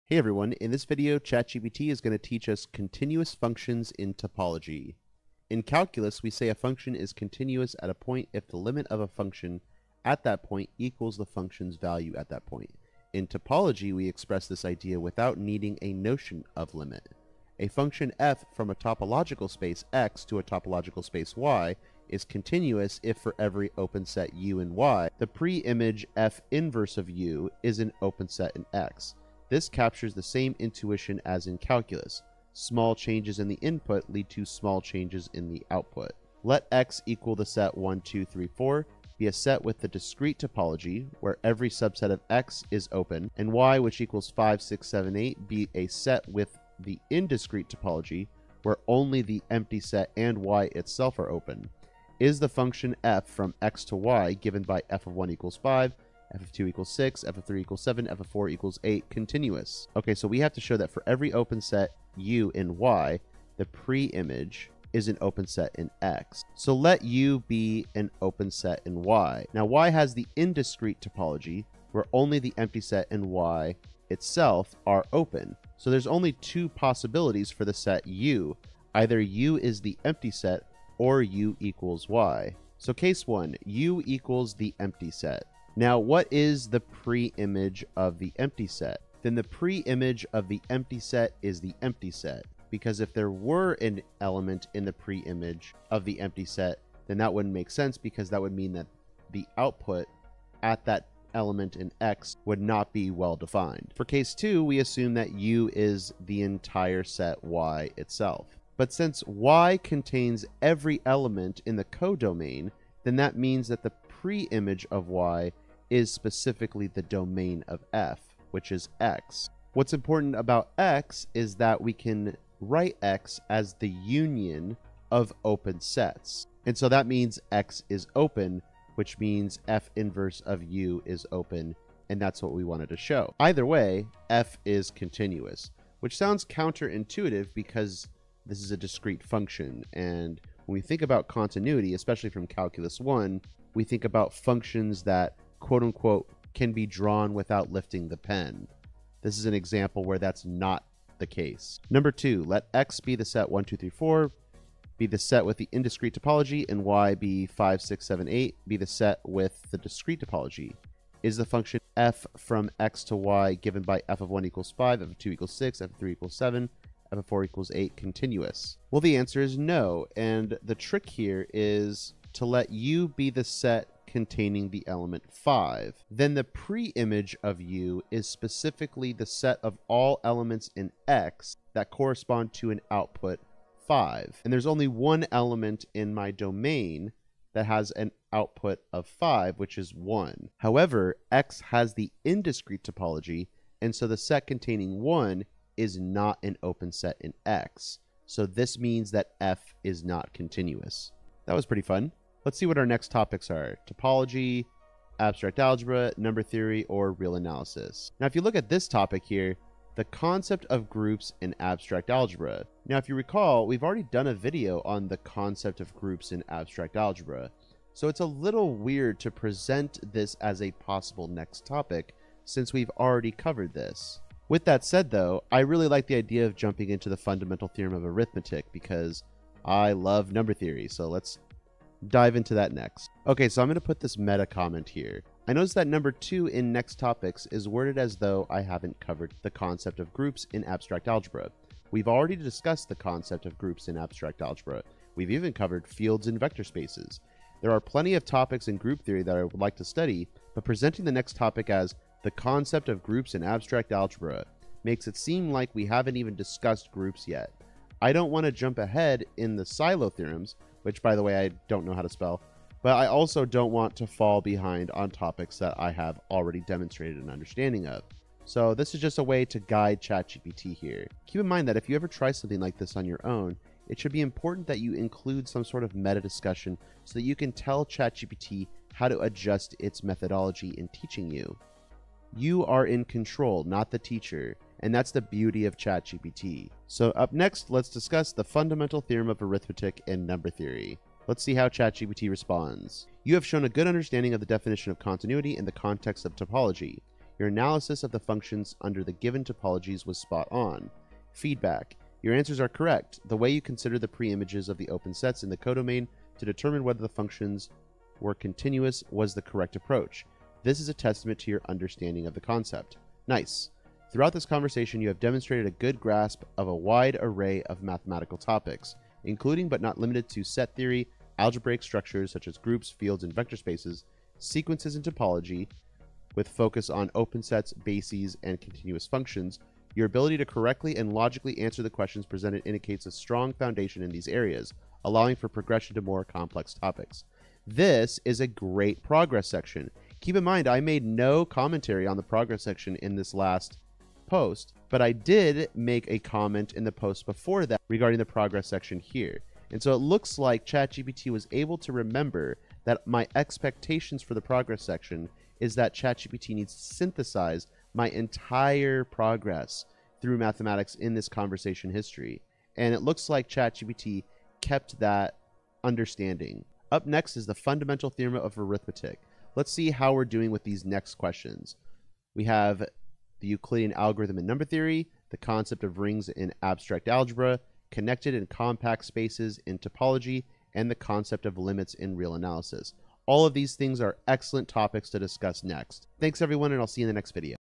Continuous Functions in Topology MathGPT Lesson 13
Hello everyone! In this lesson, ChatGPT will be guiding us through the concept of continuous functions in topology. You might remember the notion of continuity from calculus, where a function is continuous at a point if the limit of the function at that point equals the function's value at that point. Topology expresses this concept without relying on limits. Here, a function f from a topological space X to a topological space Y is considered continuous if, for every open set U in Y, the pre-image F^-1(U) is an open set in X. This maintains the same intuition as in calculus, where small changes in input lead to small changes in output.

We will explore this with the help of two examples.

Let X be the set {1, 2, 3, 4} with the discrete topology (where every subset of X is open), and Y be the set with the indiscrete topology (where only the empty set and Y itself are open). Is the function f from X to Y, defined by f(1)=5, f(2)=6, f(3)=7, f(4)=8, continuous? Yes, because for every open set U in Y, the pre-image F^-1(U) is an open set in X.

Conversely, if X is the set {1, 2, 3, 4} with the indiscrete topology and Y is the set {5, 6, 7, 8} with the discrete topology, is the function f from X to Y, defined by f(1)=5, f(2)=6, f(3)=7, f(4)=8, continuous? The answer is no, and the trick here lies in the nature of the topology on X.

These examples show that continuity in topology can behave counterintuitively compared to our understanding from calculus. In the next lesson, we're diving into the fundamental theorem of arithmetic - an interesting topic from number theory. See you there!